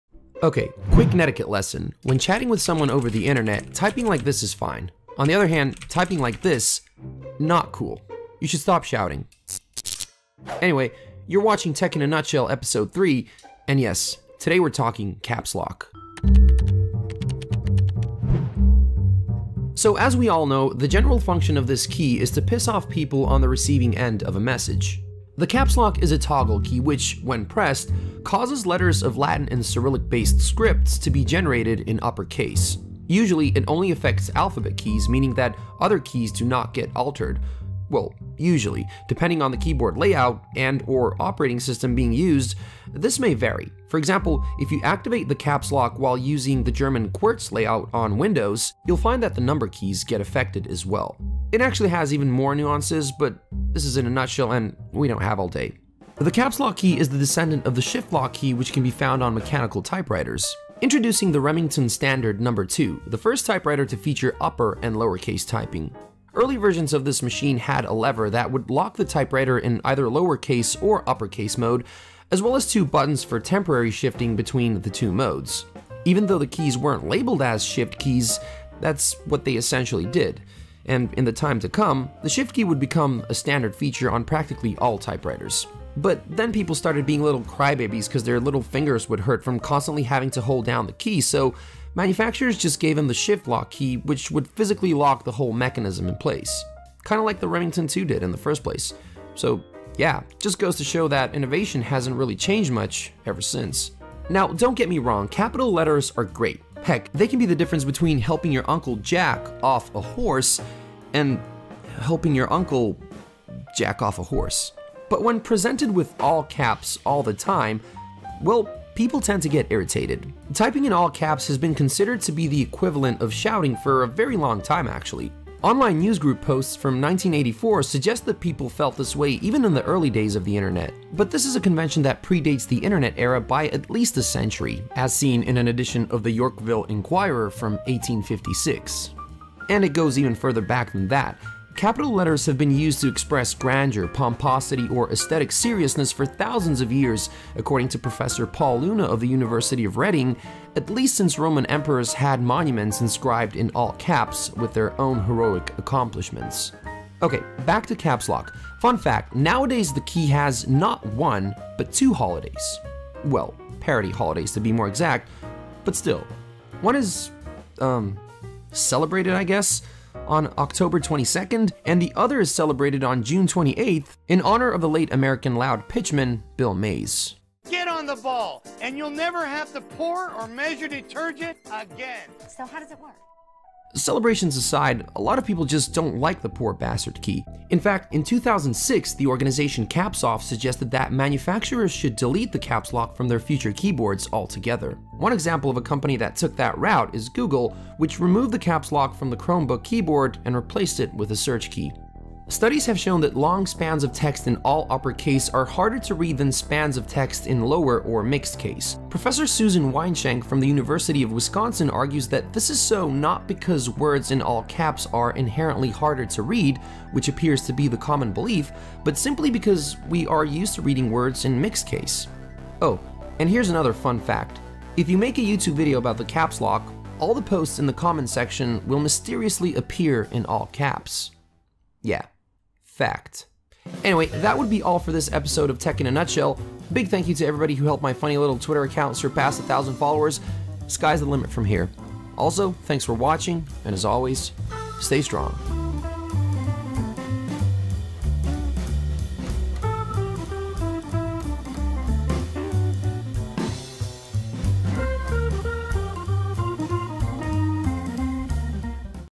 A CAPS LOCK Video
The caps lock is a toggle key which, when pressed, causes letters of Latin and Cyrillic based scripts to be generated in upper case.

The key is the descendant of the shift lock key which can be found on mechanical typewriters.

Typing in all caps has been considered to be the equivalent of shouting for a very long time now. And this is a convention that predates the age of the internet by heaps.

Nowadays, the caps lock key has two parody holidays, and they're celebrated on October 22nd, and June 28th.

Sources and references:

- The New Republic: "How Capital Letters Became Internet Code for Yelling"

- Type-writer.org: "1889 Remington Standard No. 2"

- TNW: "Today is INTERNATIONAL CAPS LOCK DAY, but what is it?"

- The Team W Blog: "100 Things You Should Know About People: #19 — It’s a Myth That All Capital Letters Are Inherently Harder to Read"

(Example of all caps being equated to the act of yelling)

- National Day Calendar: "INTERNATIONAL CAPS LOCK DAY – June 28 or October 22"